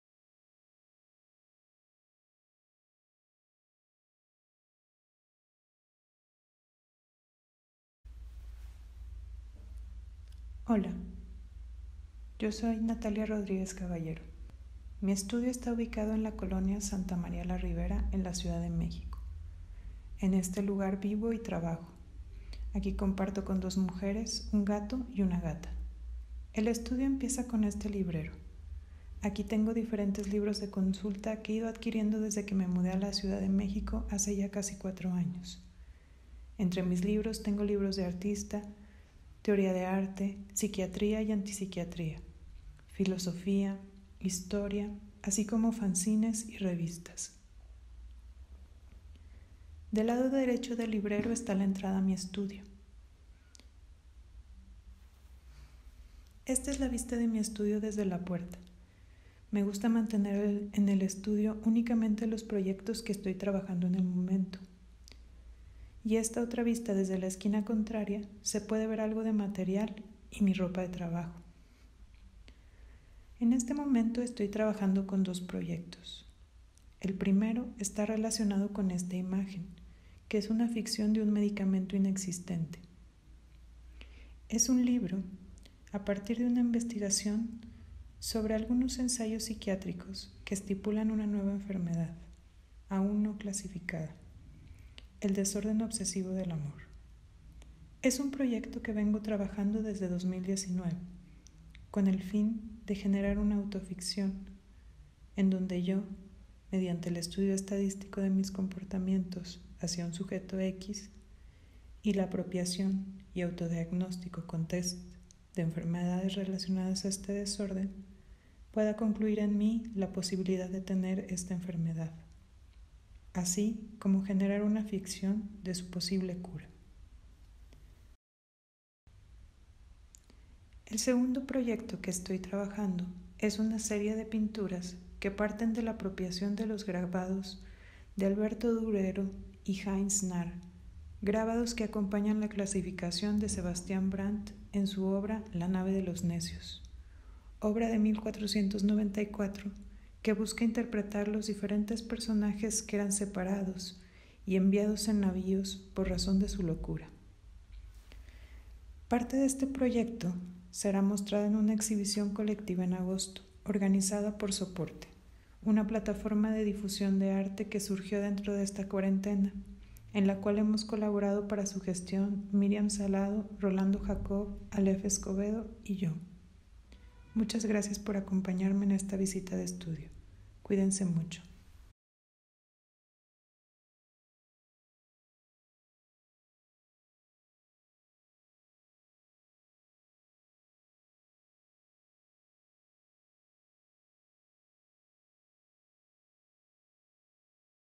Visita mi estudio Natalia Rodríguez Caballero
Desde la colonia Santa María la Ribera en la Ciudad de México la artista visual Natalia Rodriguez Caballero no recibe en su estudio y desde ahí nos muestra los proyectos en los que esta trabajando actualmente en los que se apropia de métodos de tratamiento psiquiátricos para subrayar sus contradicciones y descontextualizar los modos de operar de la medicina.